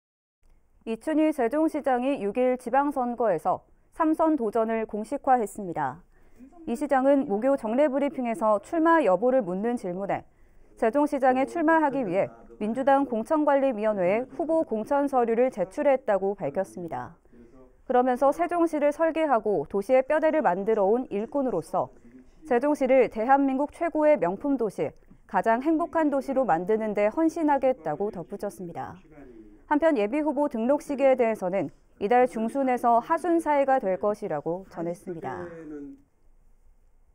[B tv 세종뉴스]이춘희 세종시장, 지방선거 3선 도전 공식화
이춘희 세종시장이 6.1 지방선거에서
3선 도전을 공식화했습니다.

이 시장은 목요 정례브리핑에서 출마 여부를 묻는 질문에
“세종시장에 출마하기 위해 민주당 공천관리위원회에
후보 공천 서류를 제출했다”고 밝혔습니다.

그러면서 “세종시를 설계하고
도시의 뼈대를 만들어온 일꾼으로서
세종시를 대한민국 최고의 명품도시,
가장 행복한 도시로 만드는데 헌신하겠다”고 덧붙였습니다.

한편, 예비후보 등록 시기에 대해서는
이달 중순에서 하순 사이가 될 것이라고 전했습니다.